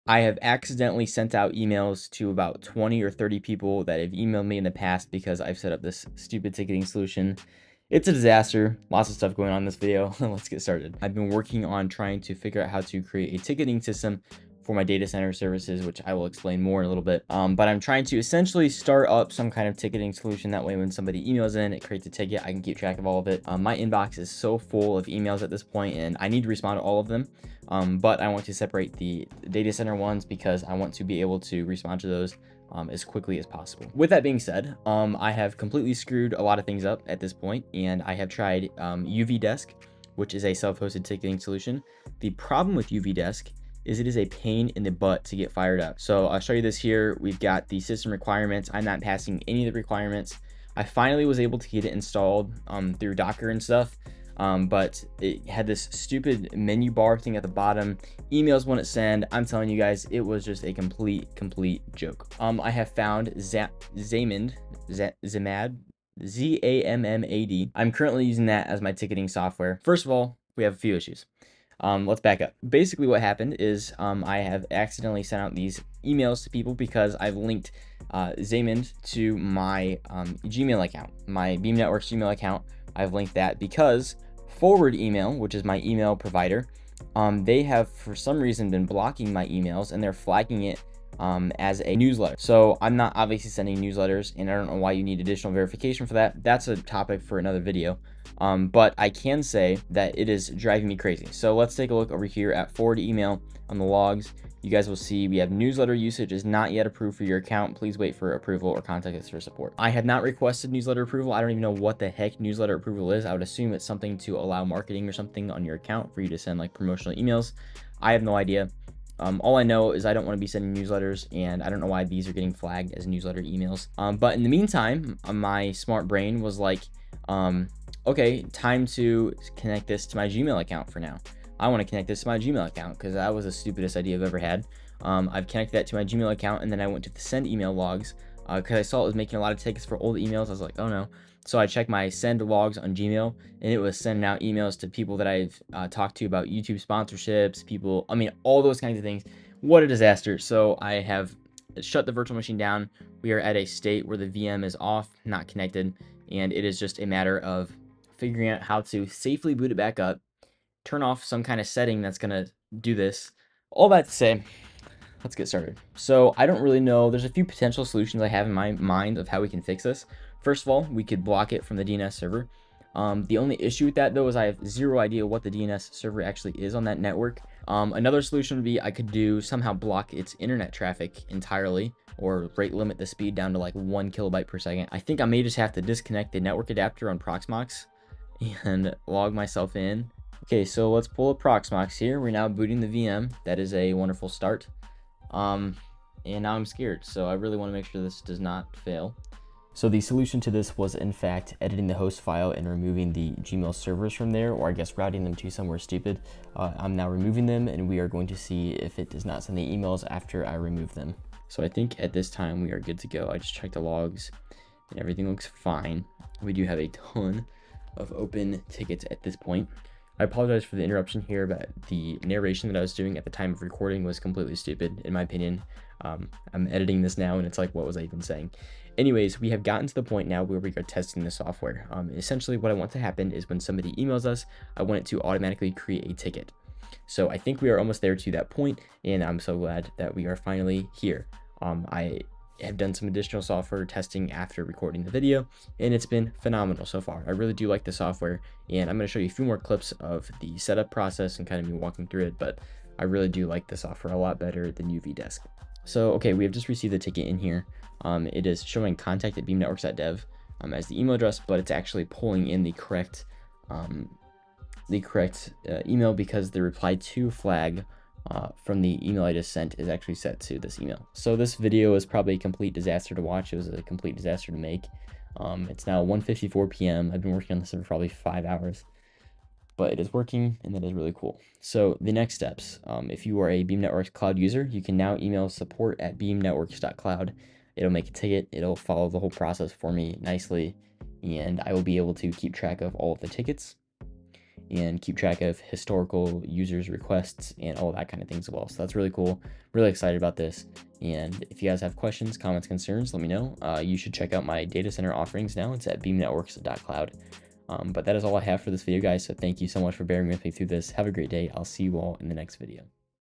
Self-Hosted Ticketing Softwares are a MESS
Enjoy this goose-chase of a video where I try to install a self-hosted ticketing solution for my datacenter company. This is a mess on so many levels, but we got it working.. That's what matters, right?

Interested in IT Consulting, or just have something to tell us? Shoot us a message!

Timestamps:
0:00 - Intro
0:33 - Messing things up
0:37 - uvdesk
1:02 - Zammad
1:12 - What happened
1:24 - ForwardEmail.net
2:03 - What went wrong
4:41 - Next steps / recap
5:00 - Conclusion

08162025